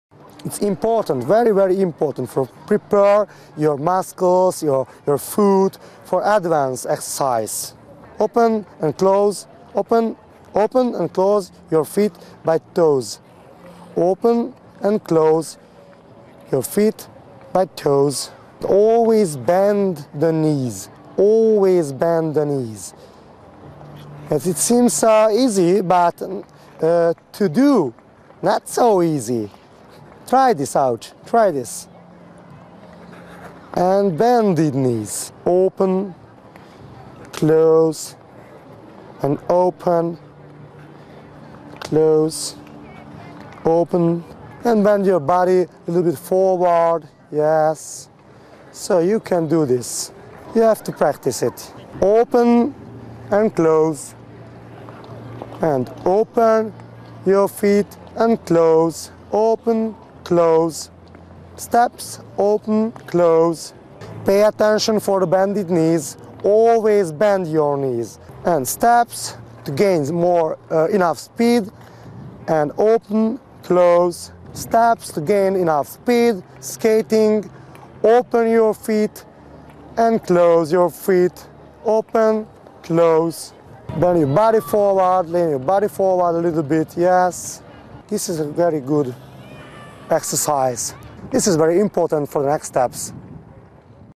Ice Skating Foot Movement
Ice Skating Foot Movement. Part of the series: Ice Skating Basics. Ice skating emphasize proper foot movement.  Learn how to move and pivot your skates from our ice skating expert in this free video clip.